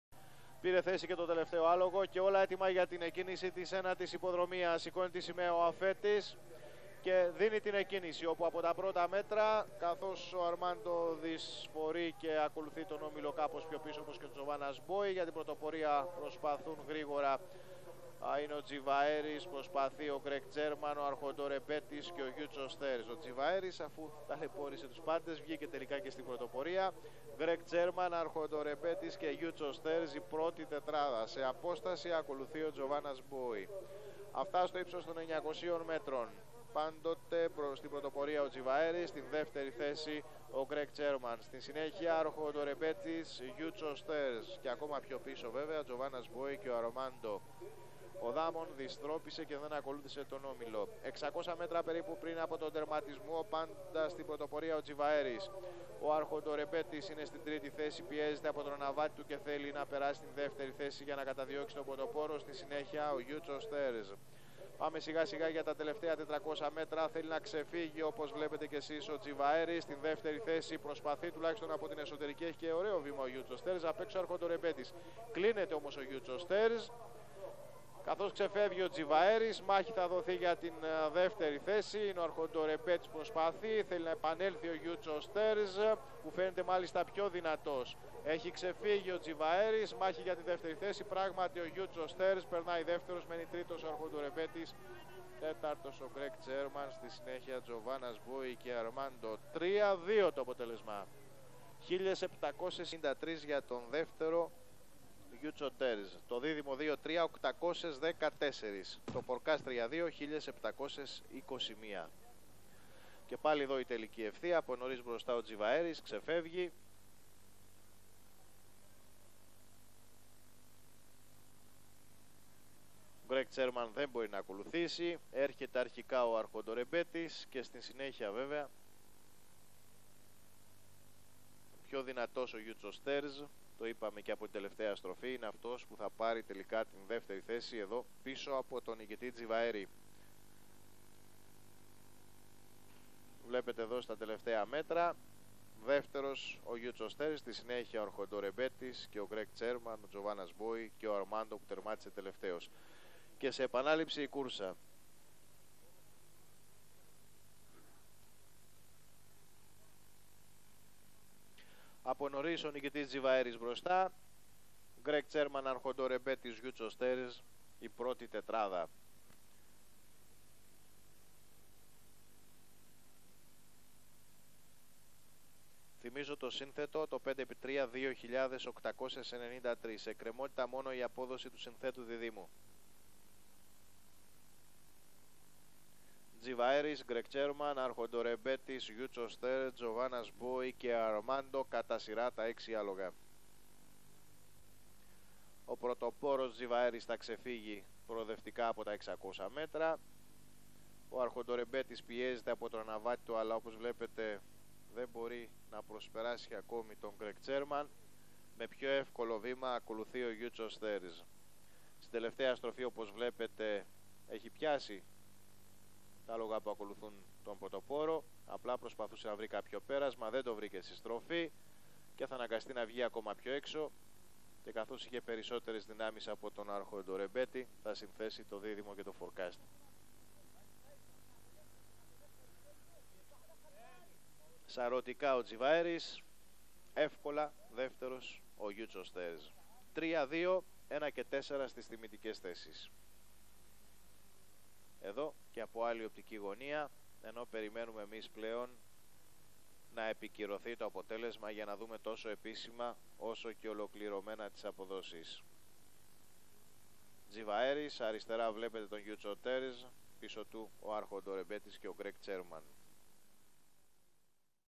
Τζιβαέρης -Γιάννης Λαζαρίδης
19 Φεβρουαρίου 2001
1ος Τζιβαέρης -Γιάννης Λαζαρίδης
2ος Γιούτσος Τέρζ-Φίλιππας Μπαριάμογλου
3ος Αρχοντορεμπέτης-Τάκης Σταύρου
Ιδιοκτησίας:Στάβλοι Β.Β.Β.-προπονητής:Μαρίνος Σαντοριναίος